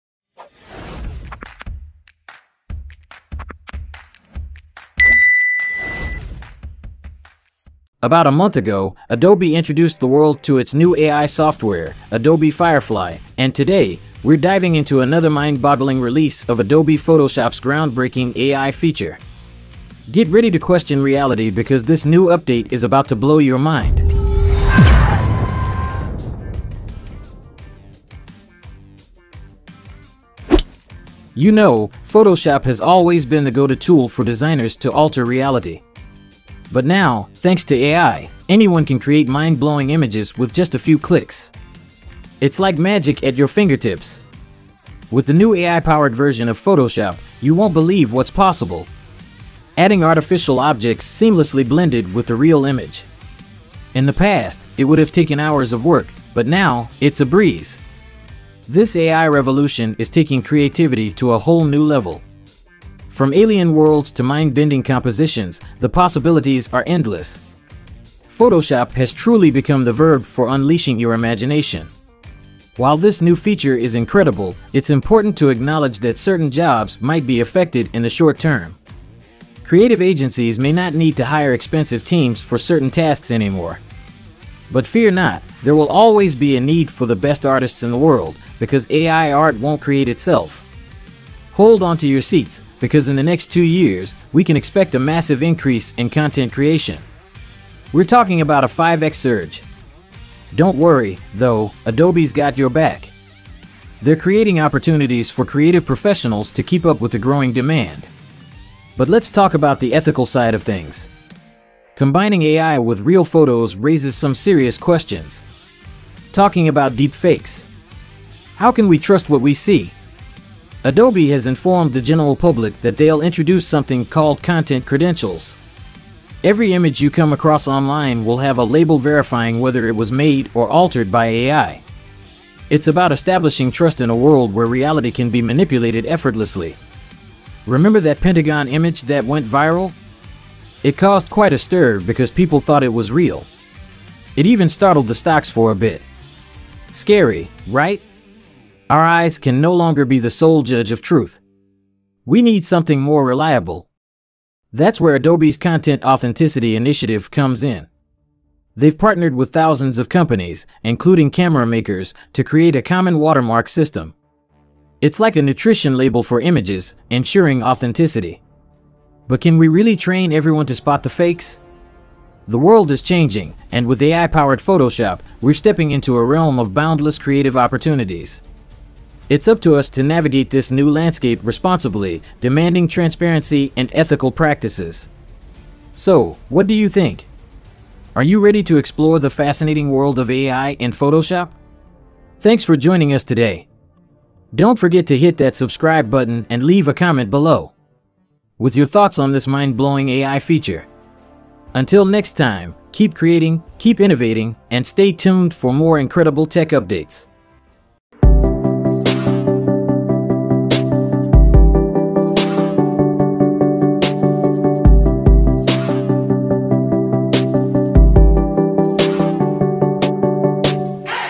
The New Photoshop is AI-Powered: Transform Reality In Seconds!
We all know how much Photoshop has changed the digital design landscape. It's been an incredible tool for manipulating reality and creating stunning visuals. Now, Adobe has taken things to the next level with a groundbreaking AI feature.

Let's take a look at what this new update has to offer.